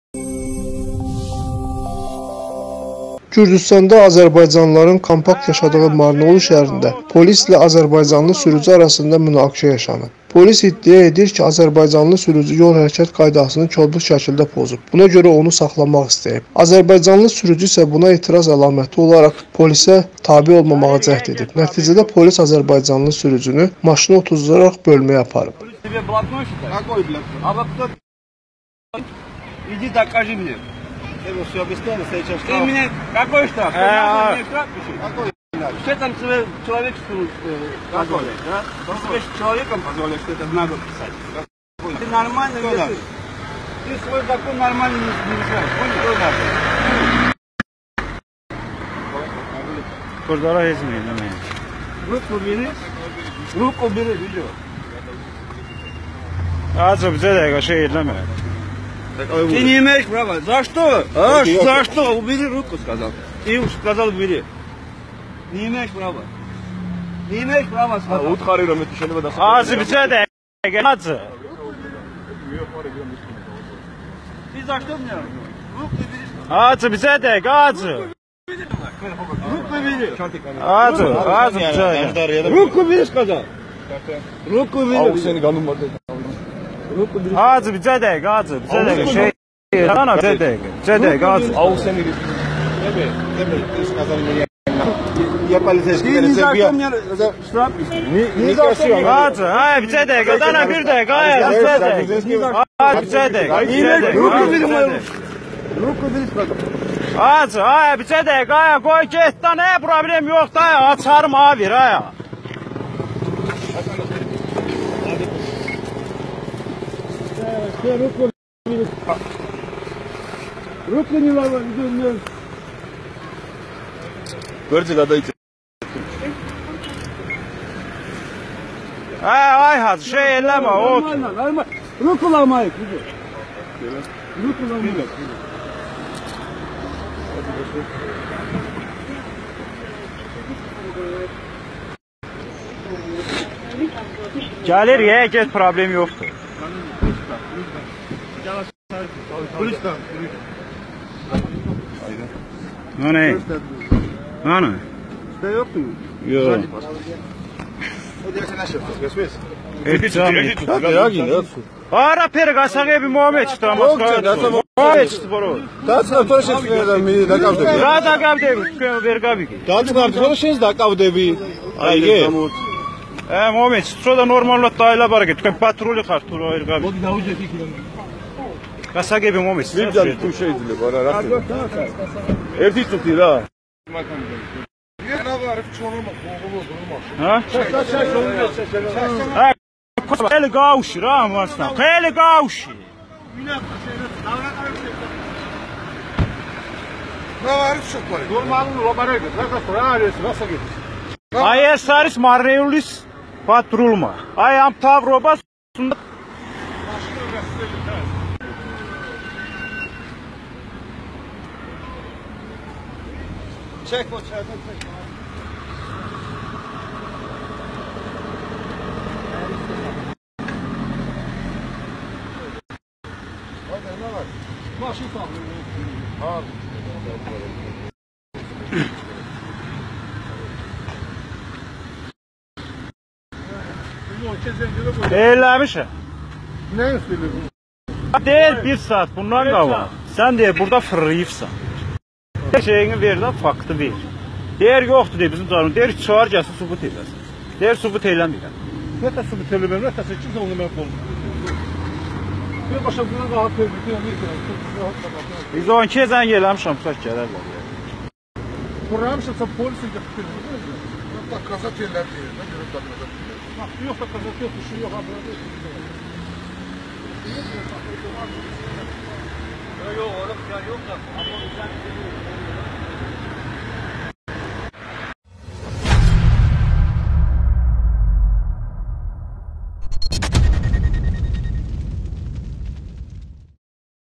Грузинские полицейские против азербайджанцев
Грузинские полицейские задержали азербайджанца.